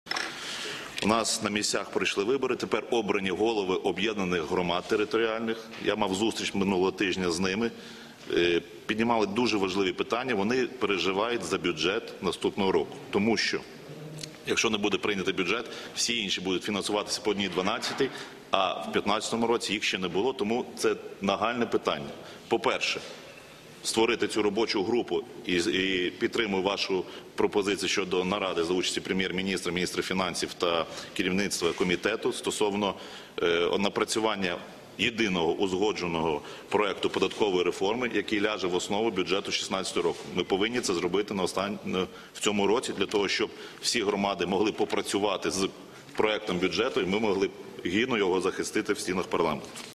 В основі Держбюджету 2016 повинен бути єдиний проект податкової реформи
Фракція "Народний фронт" підтримує пропозицію про створення робочої групи разом з Урядом і народними депутатами для напрацювання єдиного проекту податкової реформи, який ляже в основу Державного бюджету 2016 року.

Про це заявив голова парламентської фракції "Народний фронт" Максим Бурбак під час Погоджувальної ради у понеділок, 23 листопада.

Він зазначив, що від вчасного затвердження Державного бюджету найбільше залежать новостворені об’єднані територіальні громади.

"Тепер обрані голови територіальних громад піднімають важливі питання. Вони переживають за бюджет наступного року. Якщо не буде вчасно прийнятий бюджет – всі інші будуть фінансуватися по 1/12 за бюджетом минулого року, а в 2015 році об’єднаних територіальних громад не було. Бюджет повинен бути прийнятий в цьому році", - наголосив Максим Бурбак.

Він зазначив, що територіальним громадам необхідно буде час для вивчення і обговорення проекту бюджету, після чого парламент зможе за нього проголосувати.

Приєднуйся!